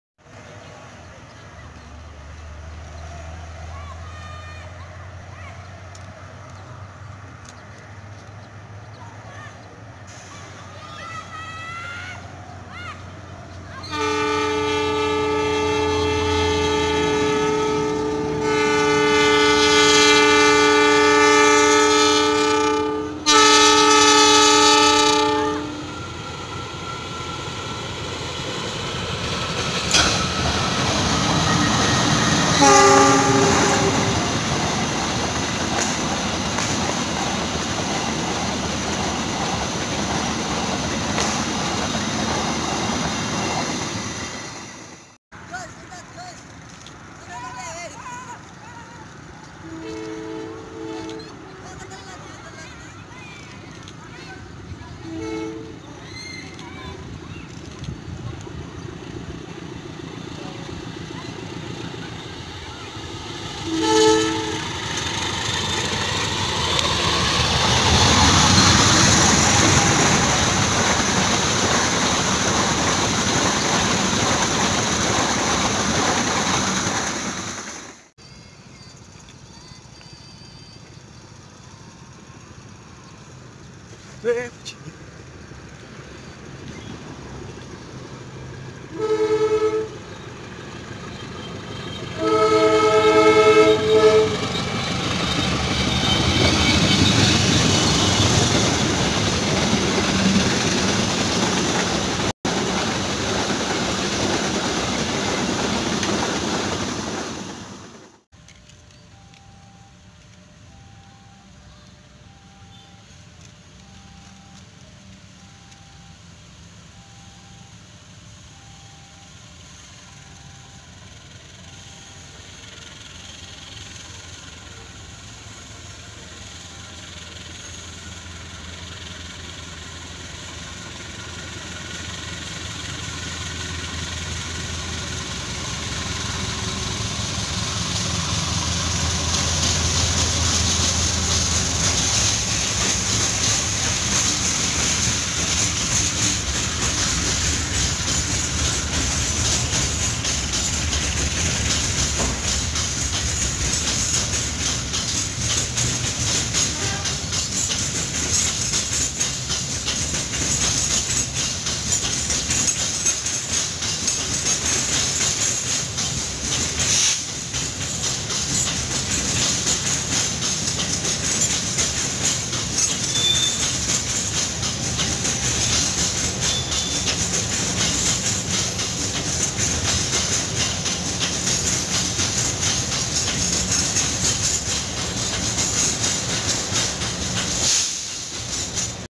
Hunting pagi di " Brexit "
Kereta api pagi yg melintas Brexit, KA Tegal Bahari, Majapahit, Matarmaja Lebaran :)
Video hanya seadanya, semoga bermanfaat...